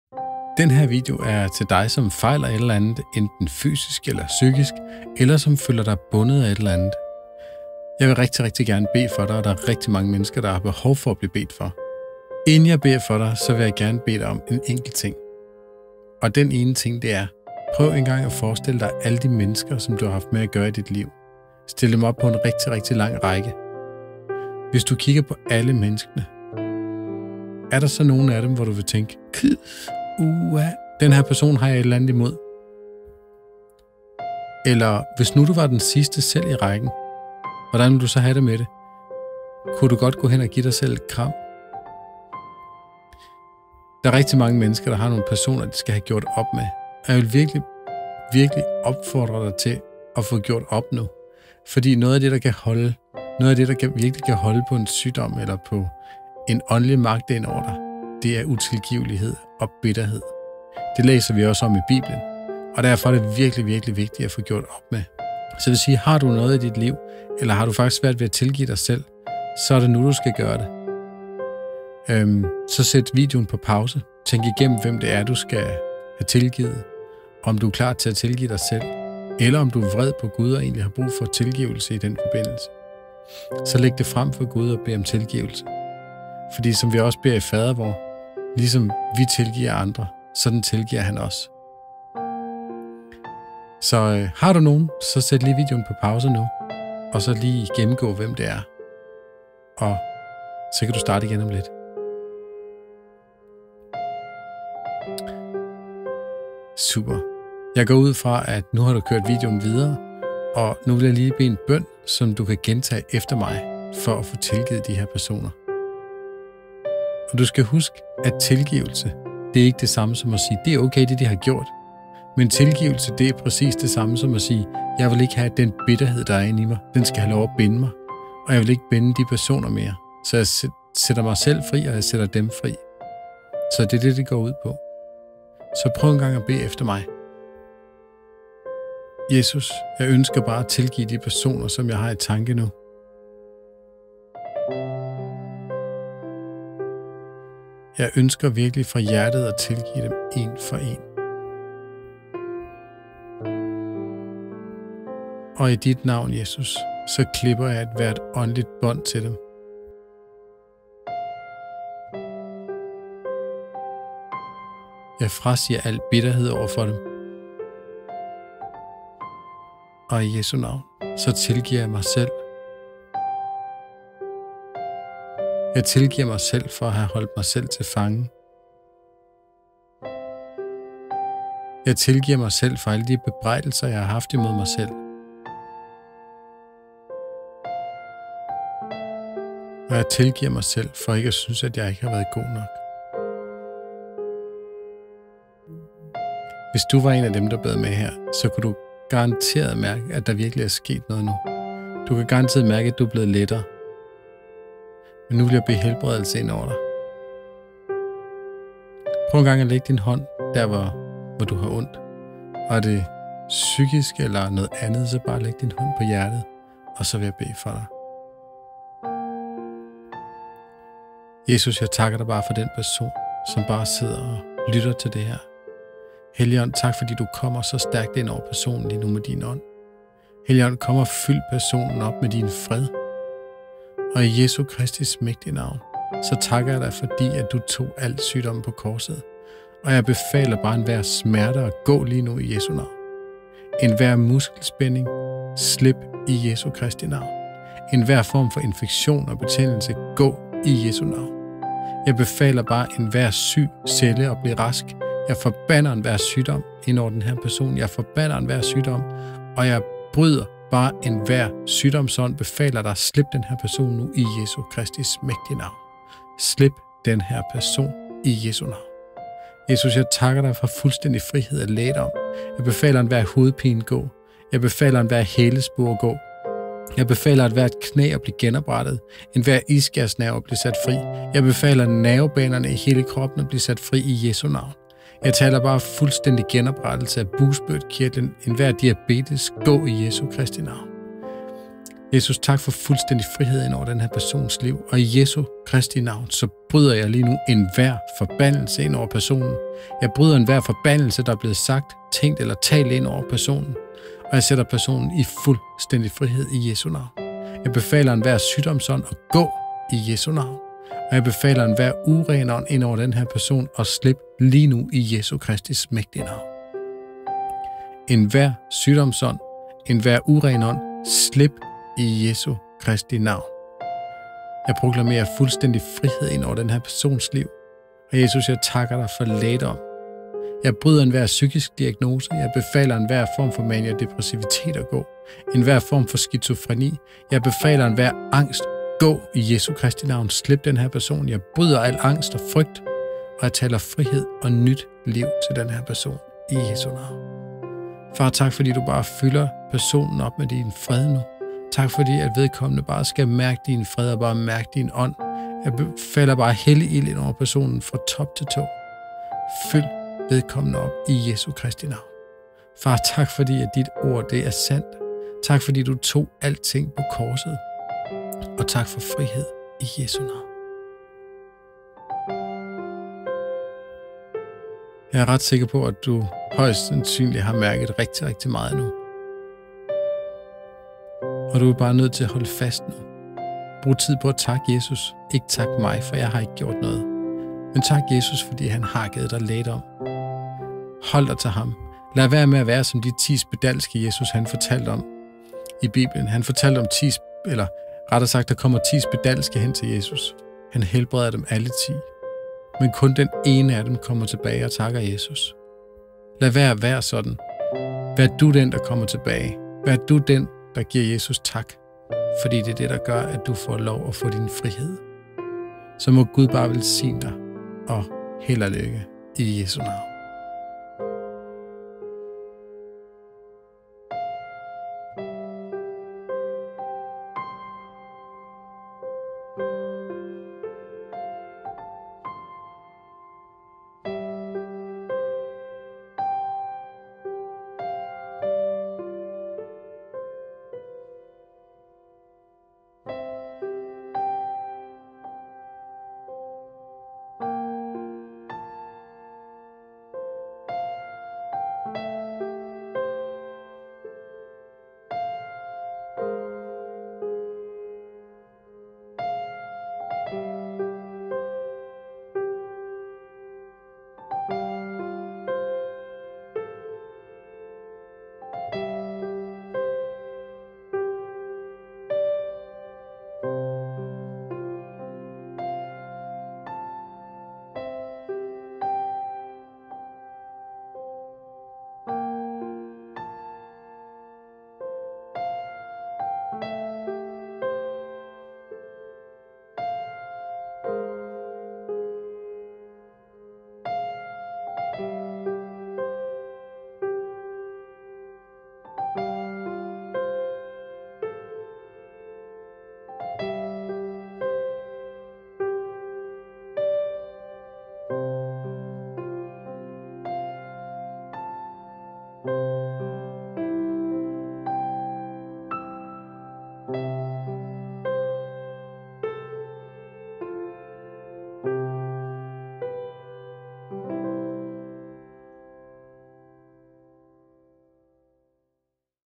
Forbøn til Jesus for helbredelse
Se denne video og bliv bedt for online lige nu. Mange mennesker oplever fysisk helbredelse igennem forbøn - også via Youtube. For Gud er nemlig ikke afhængig af, hvor og hvem der beder... men bare at du vil tage imod.